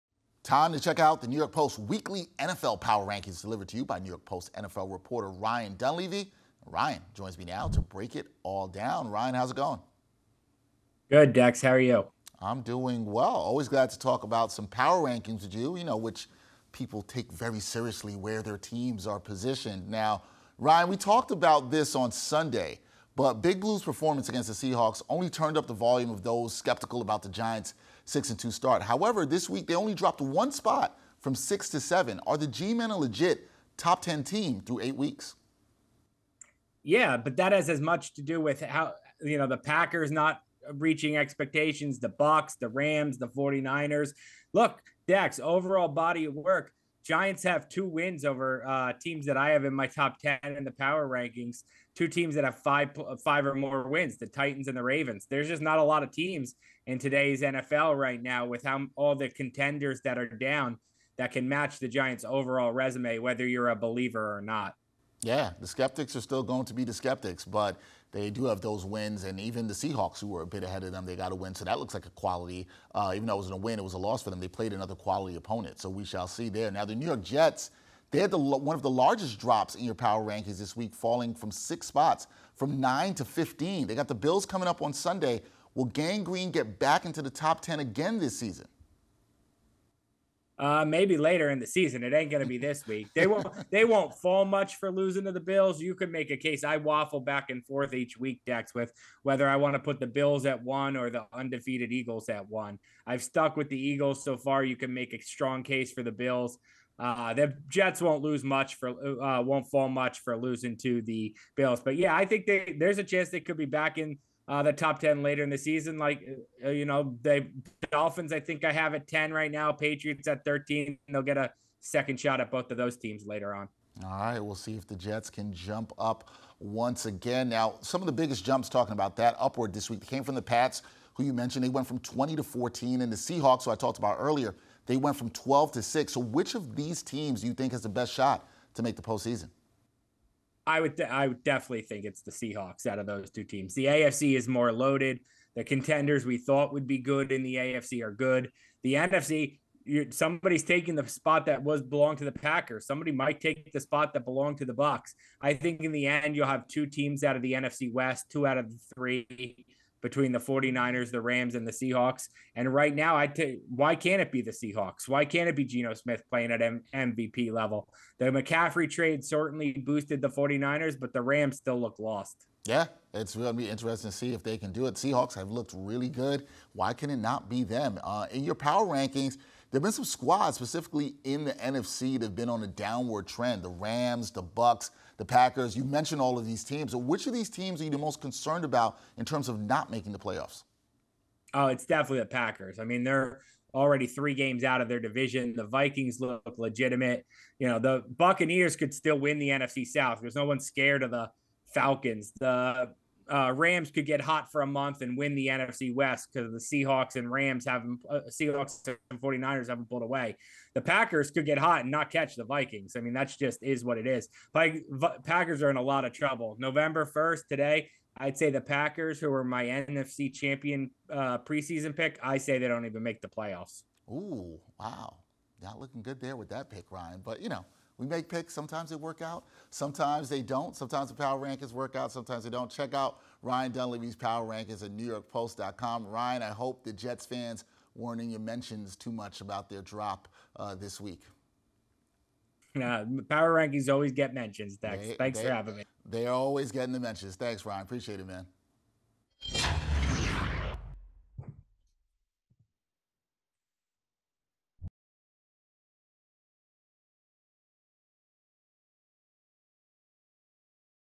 Giants, Jets rep NYC well in Post’s Week 9 Power Rankings | New York Post Sports
Ryan Dunleavy joins Dexter Henry to discuss the Giants’ and Jets’ standings in The Post’s Week 9 Power Rankings and where the rest of the NFL stands.